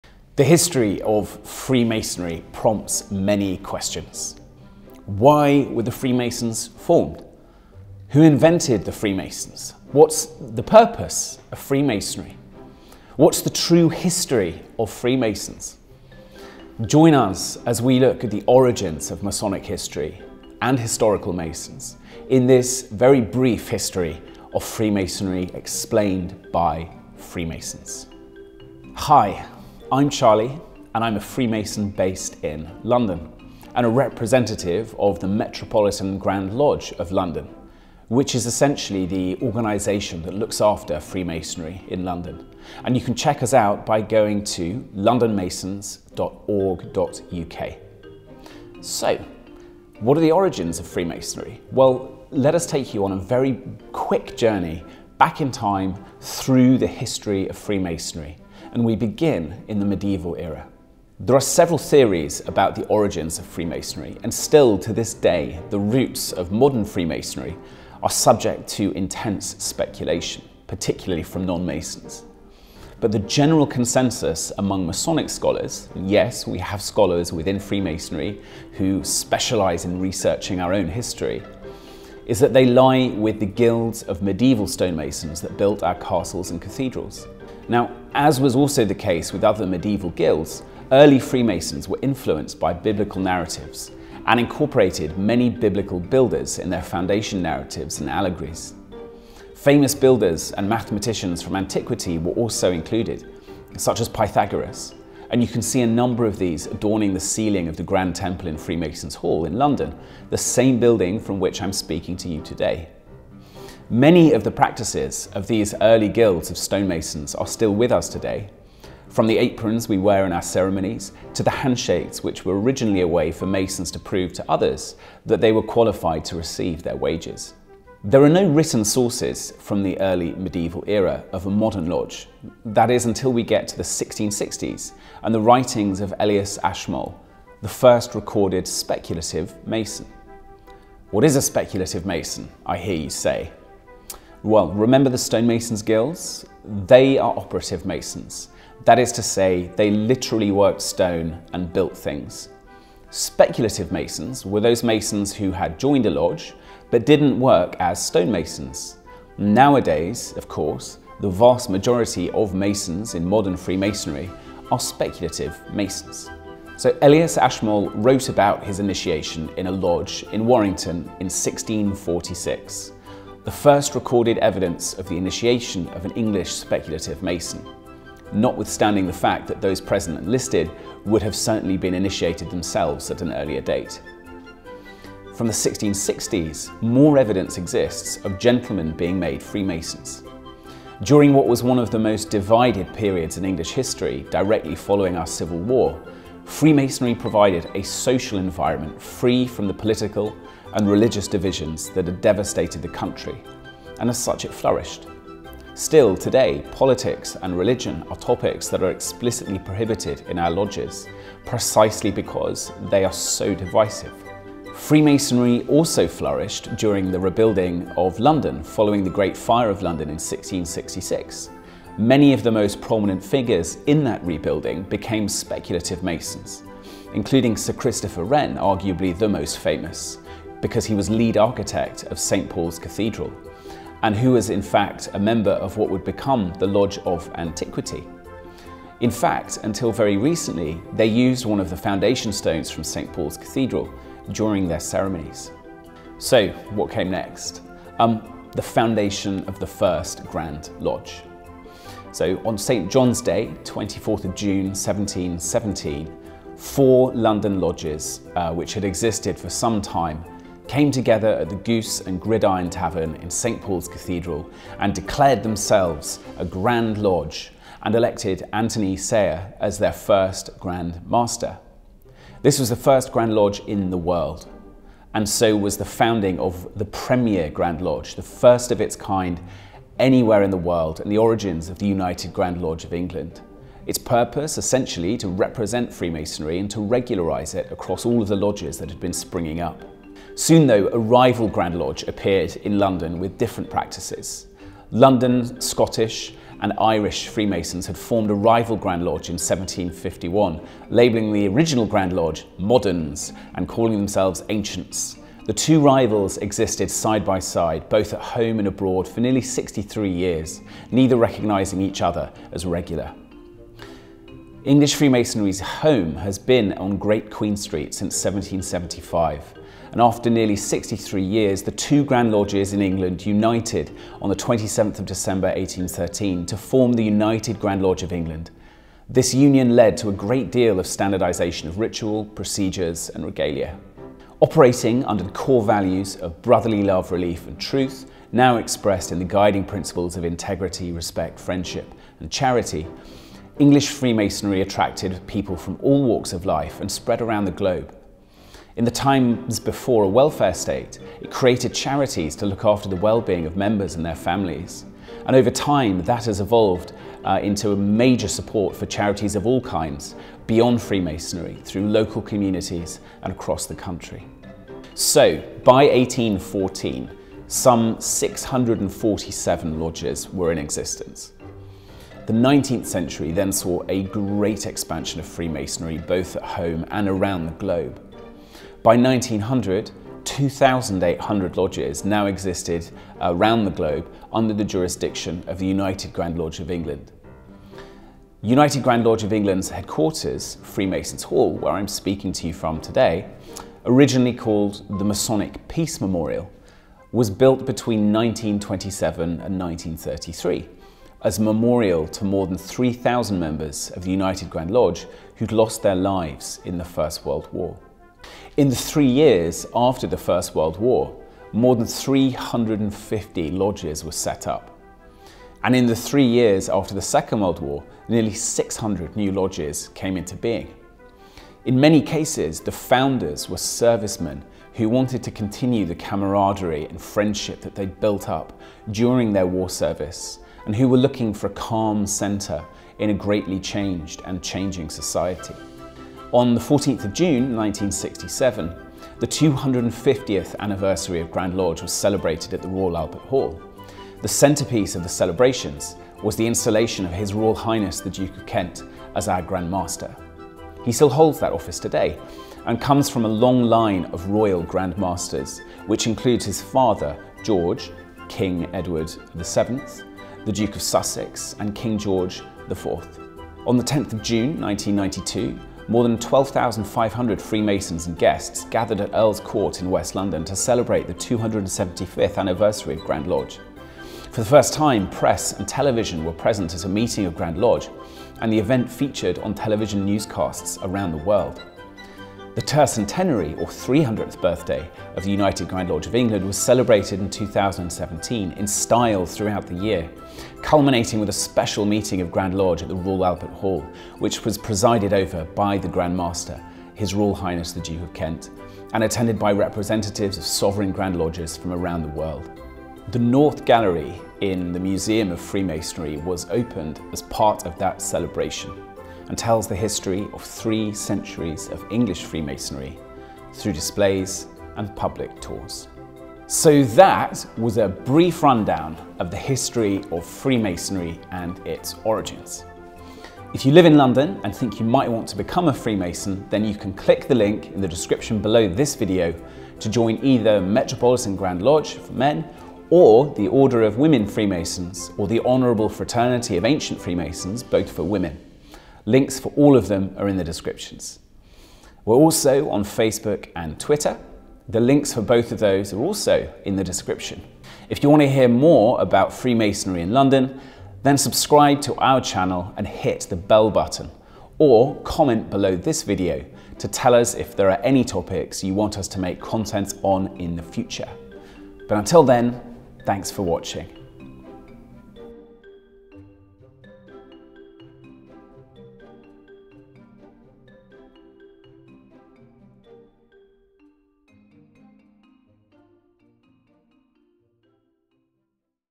The History of Freemasonry Explained (by a Freemason)
Freemasonry has a long and interesting history. In this video, Charlie a freemason from London will take you on a whistle-stop tour through the history of Freemasonry, from its mysterious origins to the modern era and the foundation of the museum of freemasonry

— CHAPTERS —
00:00 - 01:01 - Introduction
01:01 - 02:08 - The Medieval Era
02:08 - 03:34 - Elias Ashmole the first recorded "speculative mason"
03:34 - 04:06 - Sir Christopher Wren
04:06 - 04:50 - The First Grand Lodge
04:50 - 05:34 - The Formation of The United Grand Lodge of England
05:34 - 06:28 - The 19th Century
06:28 - 07:16 - Freemasonry in WW1 & WW2
07:16 - 07:43 - The Installation of the Current Grandmaster
07:43 - 08:36 - 275th and 300th Anniversaries
08:36 - 09:39 - Outro

What are the origins of Freemasonry?

There are several theories about the origins of Freemasonry and still to this day the roots of modern Freemasonry are subject to intense speculation particularly from non-Masons. But the general consensus among Masonic scholars is that they lie with the guilds of mediaeval stonemasons that built our castles and cathedrals.

Who was the first freemason recorded in history?

Elias Ashmole wrote about his initiation in a lodge in Warrington in 1646, the first recorded evidence of the initiation of an English Speculative Mason, notwithstanding the fact that those present and listed would've certainly been initiated themselves at an earlier date. From the 1660s, more evidence exists of gentlemen being made Freemasons.

When was the first Grand Lodge Formed?

On St. John's Day, 24th of June, 1717, four London lodges, which had existed for some time, came together at the Goose and Gridiron Tavern in St. Paul's Cathedral and declared themselves a Grand Lodge and elected Anthony Sayer as their first Grand Master. This was the first Grand Lodge in the world, and so was the founding of the Premier Grand Lodge, the first of its kind anywhere in the world and the origins of the United Grand Lodge of England.

Did Freemasons fight in the first world war?

The United Grand Lodge of England's headquarters, Freemasons' Hall, originally called the Masonic Peace Memorial, was built between 1927 and 1933 as a memorial to more than 3,000 members of the United Grand Lodge who'd lost their lives in the First World War. In the three years after the First World War, more than 350 lodges were set up, and in the three years after the Second World War, nearly 600 new lodges came into being. In many cases, the founders were servicemen who wanted to continue the camaraderie and friendship that they'd built up during their war service and who were looking for a calm centre in a greatly changed and changing society.

How old is Freemasonry?

The tercentenary or 300th birthday of the United Grand Lodge of England was celebrated in 2017 in style throughout the year, culminating with a special meeting of Grand Lodge at the Royal Albert Hall, which was presided over by the Grand Master, His Royal Highness the Duke of Kent, and attended by representatives of Sovereign Grand Lodges from around the world. The Tercentenary was to celebrate the Foundation of the first grand lodge founded 300 years ago.

Can I learn more about the history of Freemasonry?

The Museum of Freemasonry at Freemason’s Hall in London tells the history of three centuries of English Freemasonry through displays and public tours, you can find their